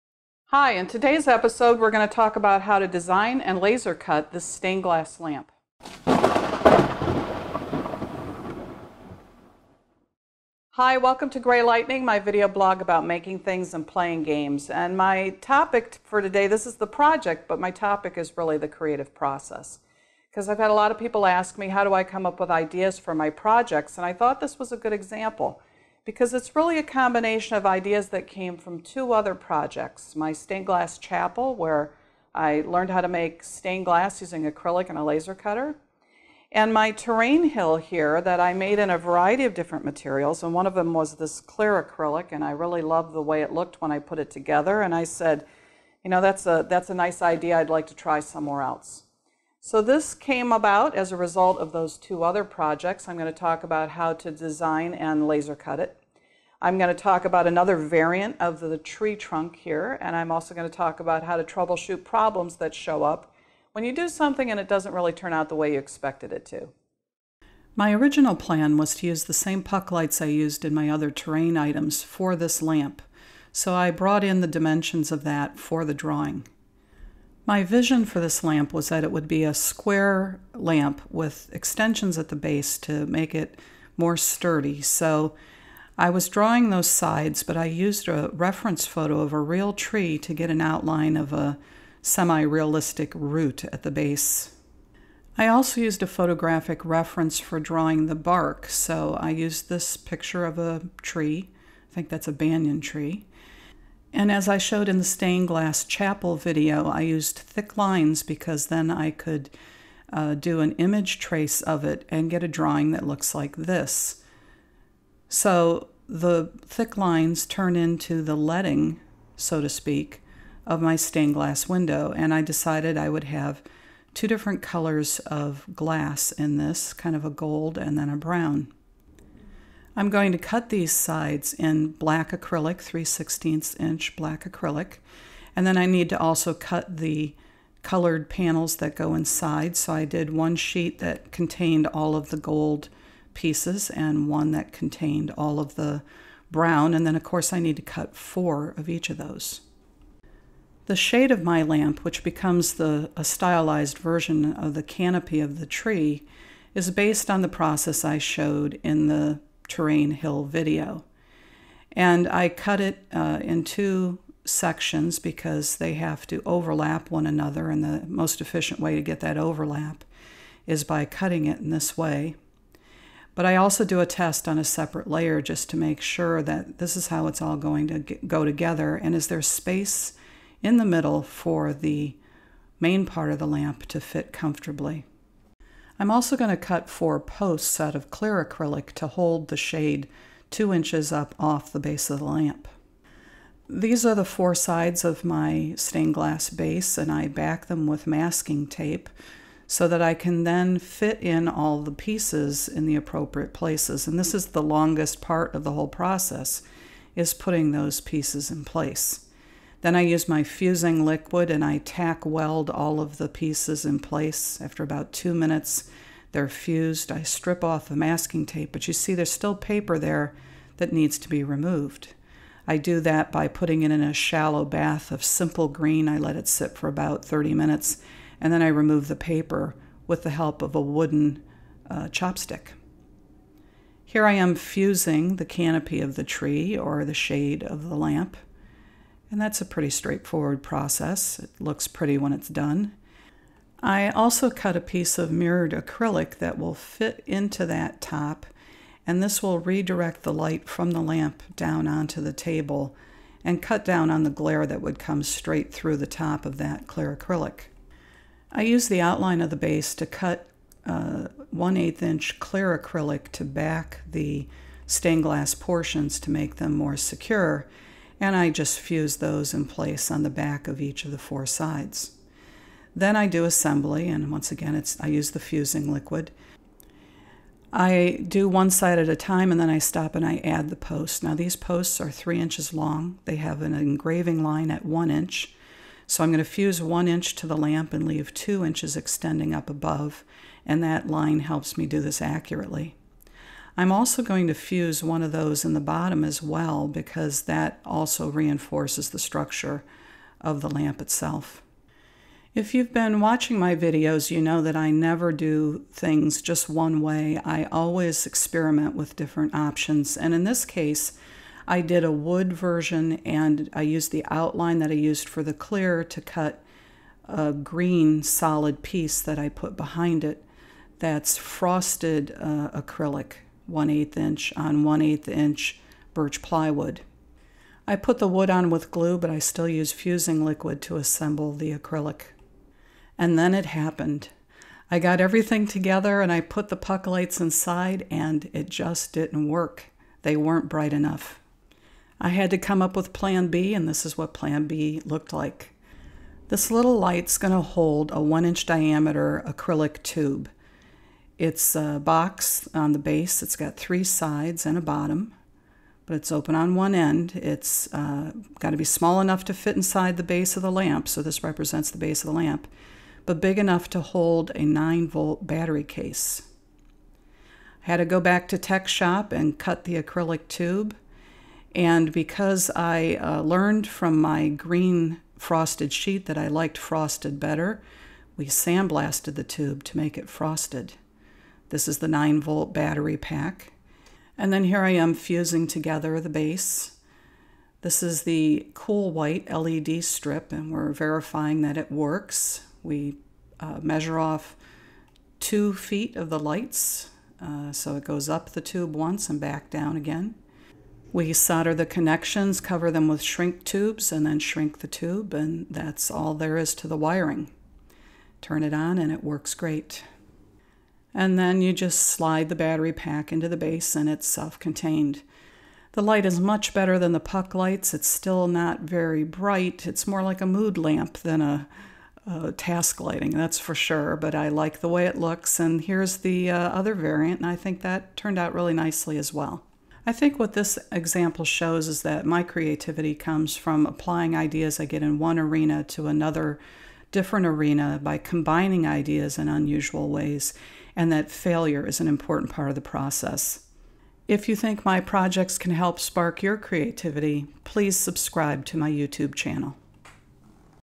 How to Design and Laser Cut a 'Stained Glass' Lamp
In this episode I show how to use Adobe Illustrator to design a 'stained glass' lamp that can be cut on a laser cutter. I show how to assemble the lamp, and how to build an alternate lighting source when the original lighting source doesn't work. I show two variations of the design and talk about how new ideas can come from combining old ideas, and from working around design challenges.